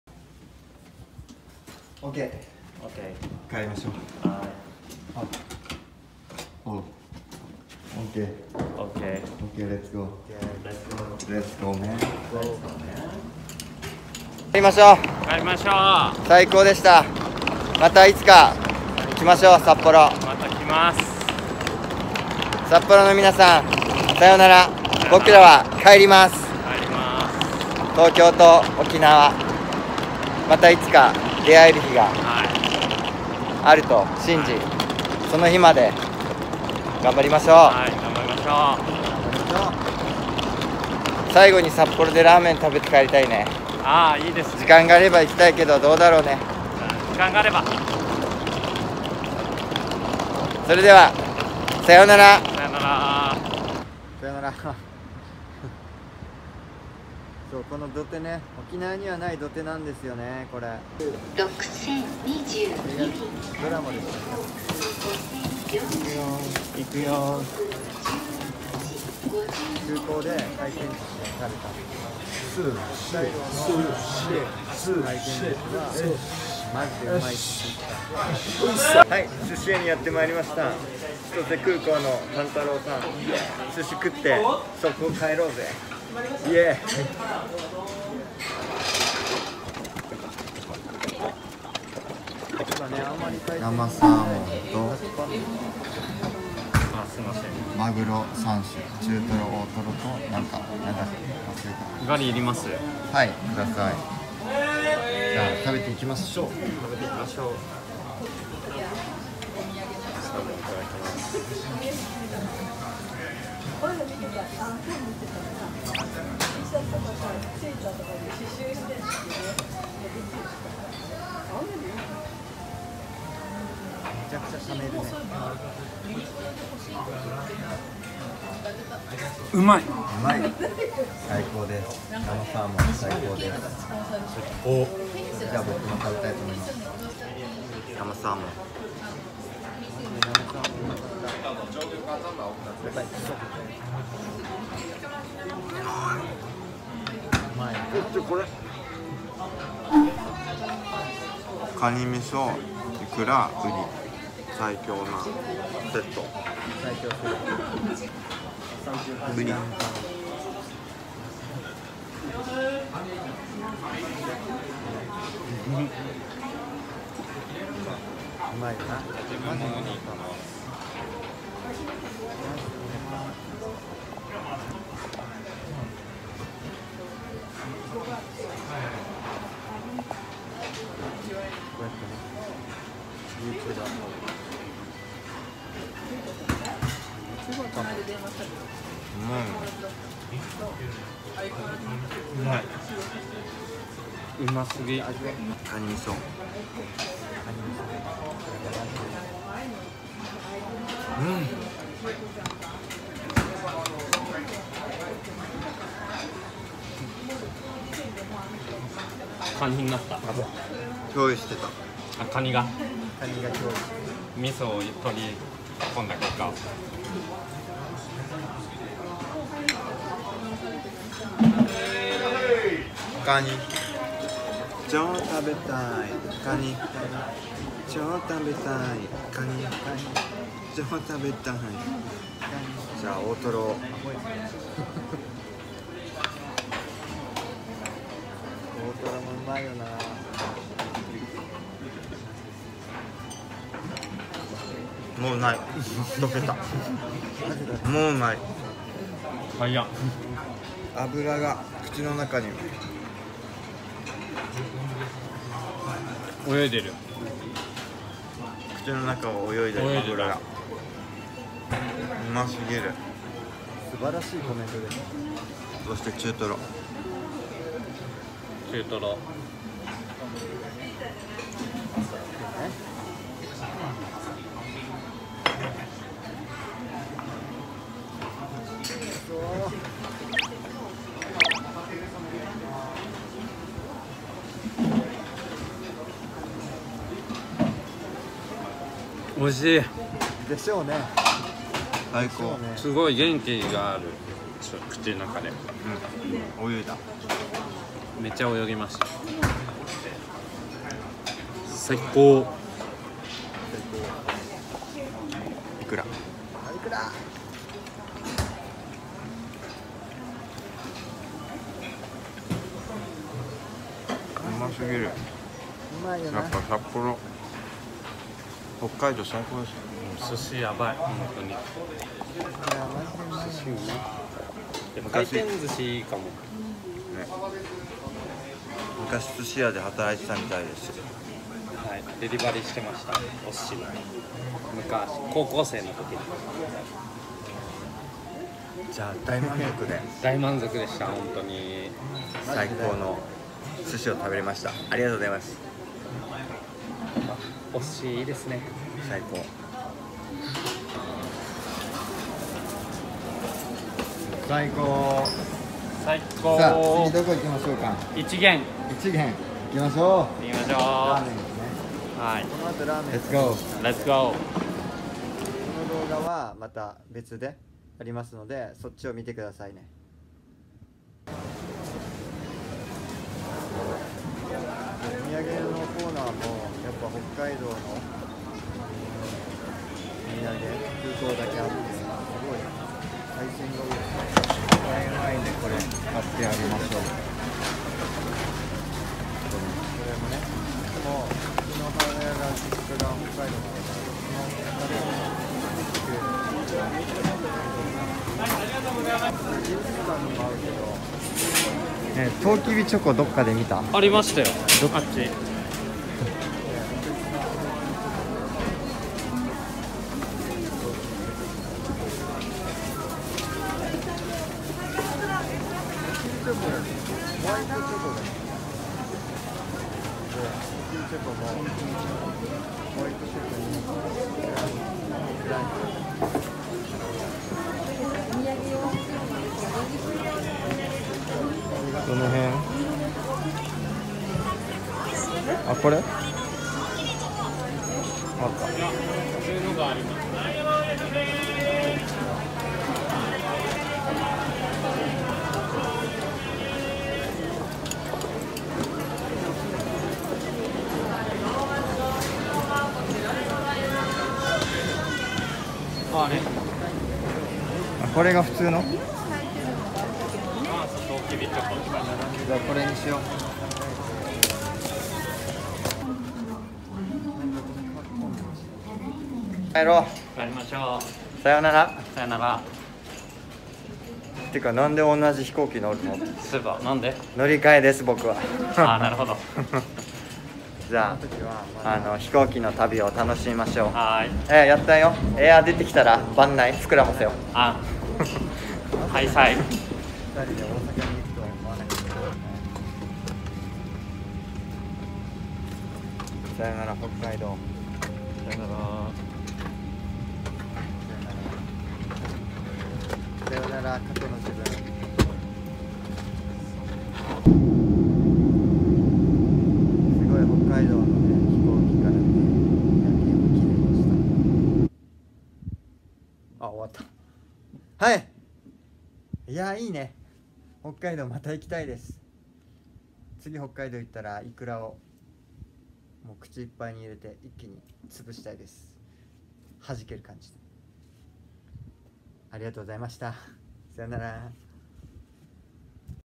北海道へ行ってきた。最終日！新千歳空港で爆食い#回転寿司#空港グルメ#北海道#大トロ
詳しくはweb shopへ

LEFLAH沖縄店
沖縄県那覇市牧志2-7-24 1F leflah沖縄店

モノレール牧志駅から徒歩30秒から5分

當銘たけしアカウント